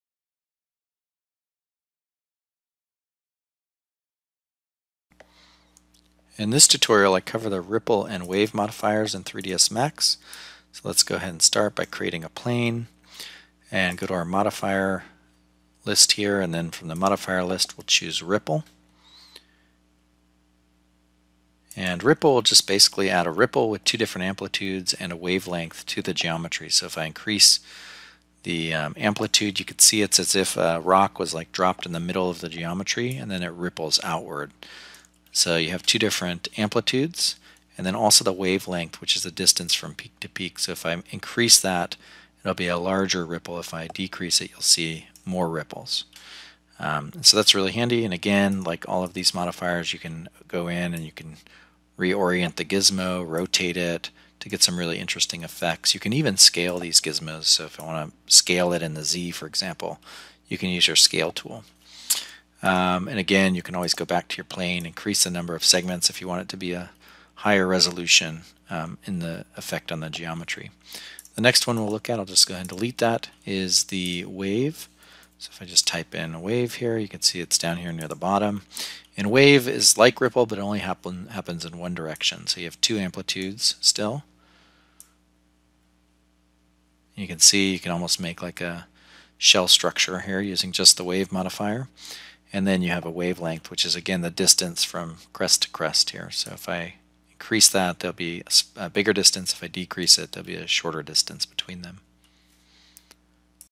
3ds Max Tutorial: Ripple and Wave Modifiers
In this tutorial, I cover the ripple and wave modifiers in 3ds Max.